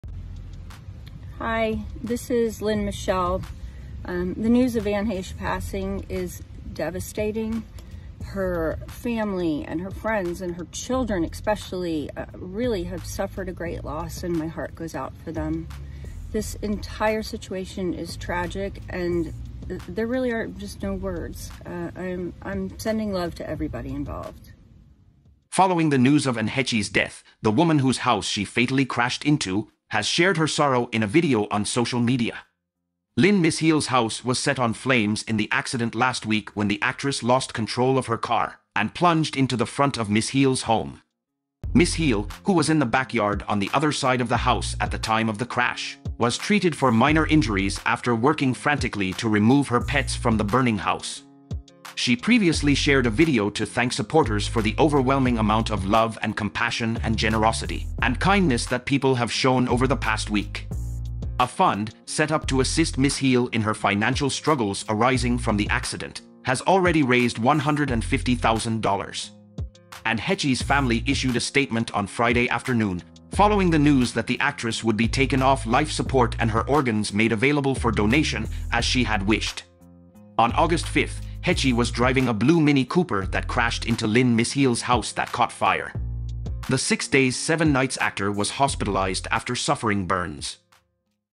Anne Heche Crash House Owner Expresses Sorrow, Thanks Supporters For Fundraising
Anne Heche Crash House Owner Expresses Sorrow, Thanks Supporters For Fundraising $150,000 - Following the news of Anne Heche's death, the woman whose house she fatally crashed into, has shared her sorrow in a video on social media.